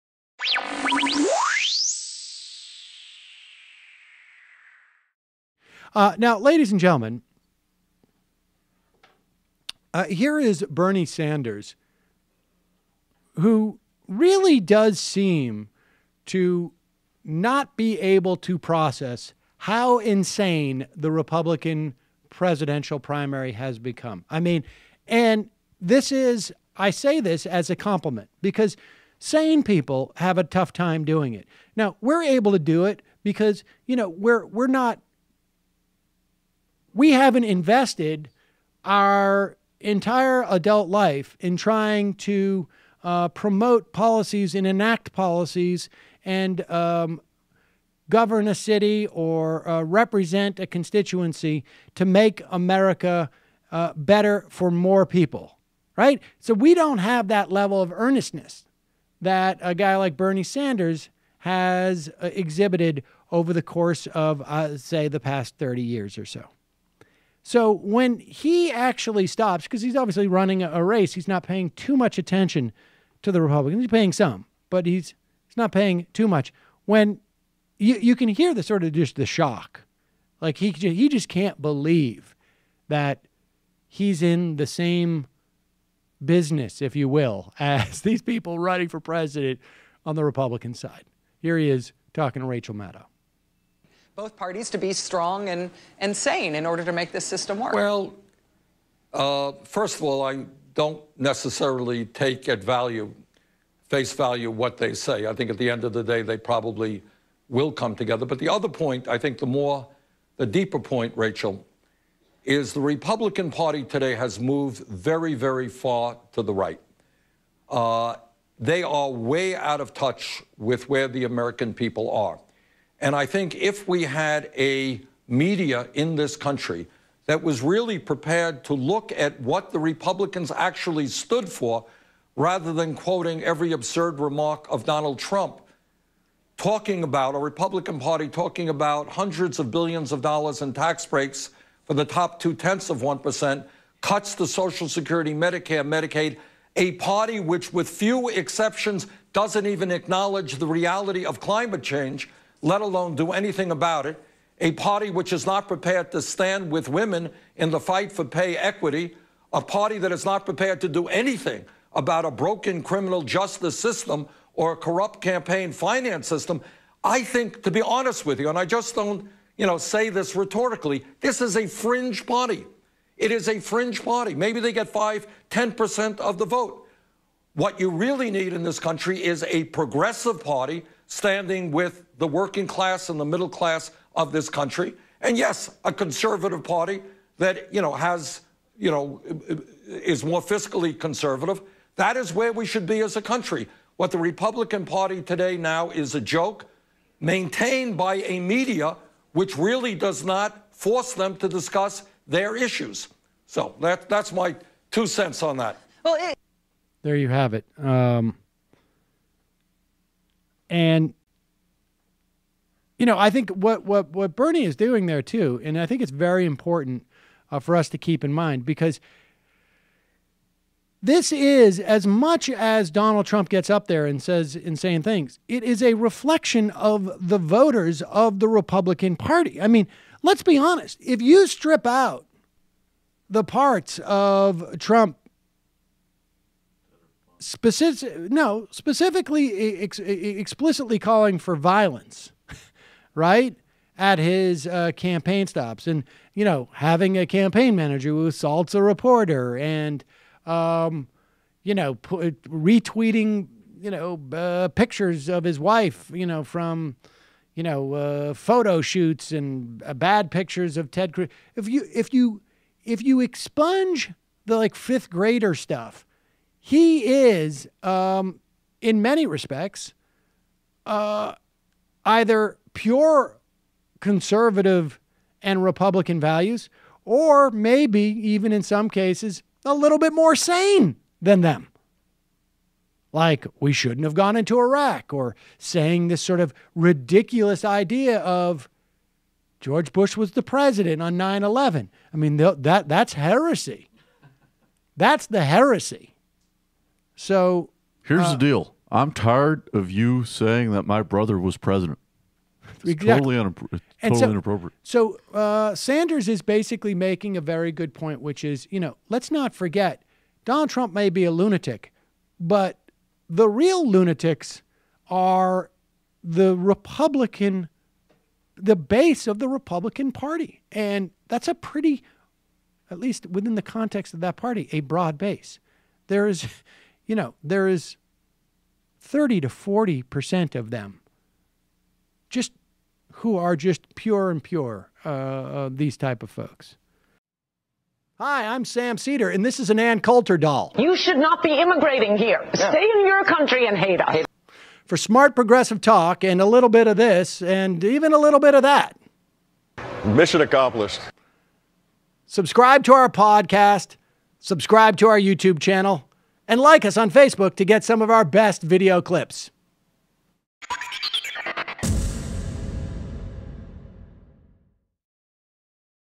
Bernie Sanders: The GOP Is a Joke Maintained By Media
Sanders says that the Republican Party has become a far-right movement, out of touch with the reality of the country, and that the media coverage of the GOP candidates' ridiculous behavior is part of the problem. He's ... right.